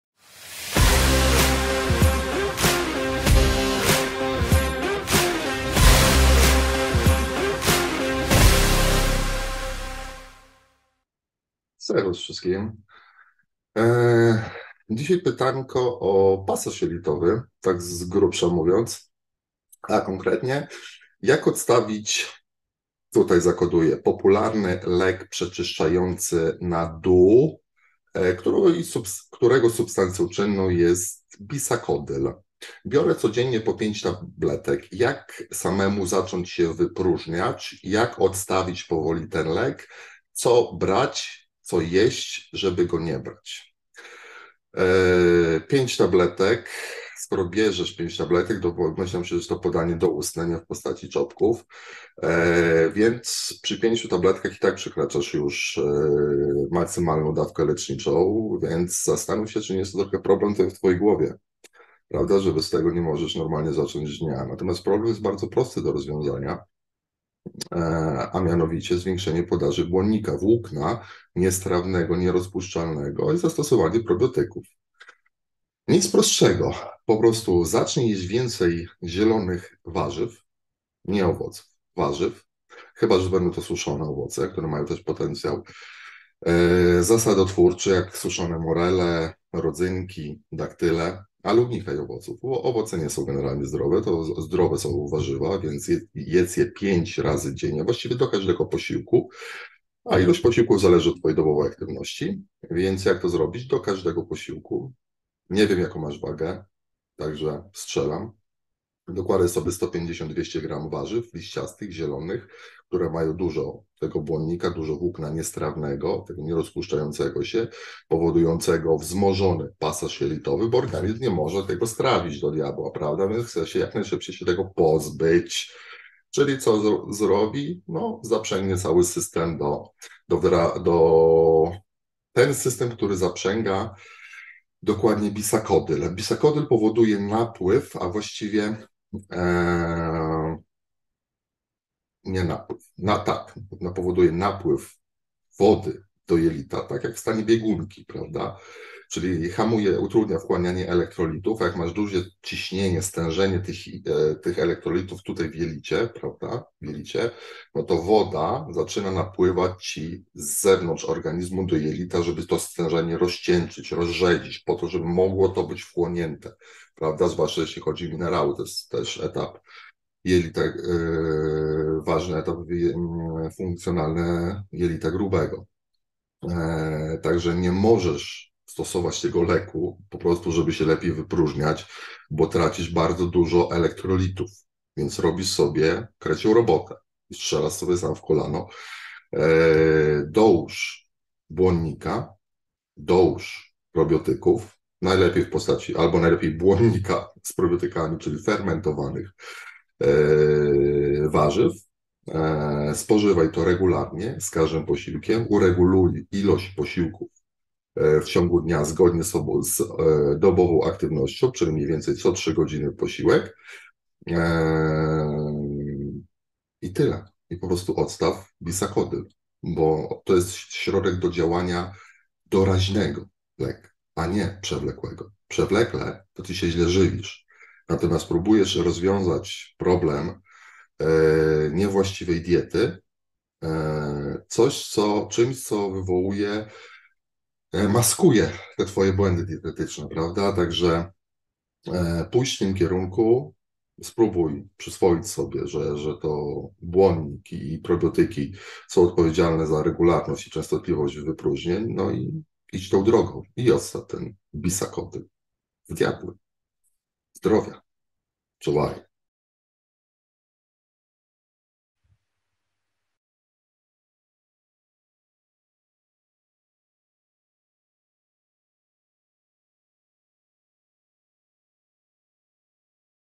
JAK ODSTAWIĆ LEK PRZECZYSZCZAJĄCY? / HOW TO LAY OFF LAXATIVES
UDOSTĘPNIJ dalej! 👊🙏

Dzisiaj pytanko o pasaż jelitowy.
„Jak odstawić popularny lek przeczyszczający na „Du…”, którego substancją czynną jest bisakodyl.
Biorę codziennie po 5 tabletek.
Jak samemu zacząć się wypróżniać?
Jak powoli odstawić ten lek?
Co brać, co jeść, żeby go nie brać…?”

Today’s question is about intestinal passage.
„How to lay off – let me code it here – a popular laxative medication starting with ‘Du’, the active substance of which is bisacodyl.
I take five tablets every day.
How can I start evacuating on my own?
How can I slowly lay off this medication?
What to take, what to eat so that I don’t have to take it?”